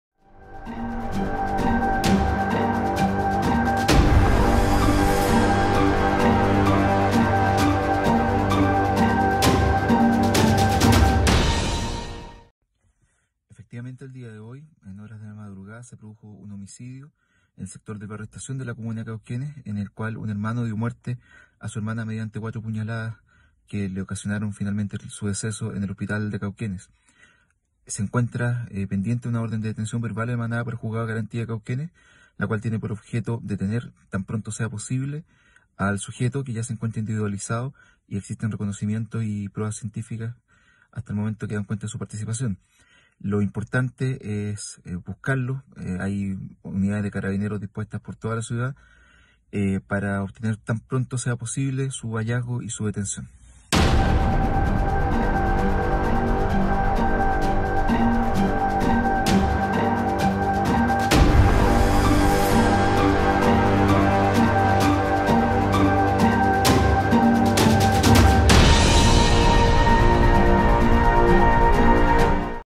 Fiscal Francisco Avila por sujeto que apuñalo a su hermana en Cauquenes
Los hechos acontecieron durante la mañana de este domingo, a eso de las 7:00 horas, cuando ambos hermanos por parte de madre -ella de 36 y él de 31- habrían sostenido una discusión de la cual -hasta el momento- se desconocen las causas. Lo que sí se sabe es que de un momento a otro todo se descontroló y el hombre propinó cuatro puñaladas a la mujer, huyendo del lugar.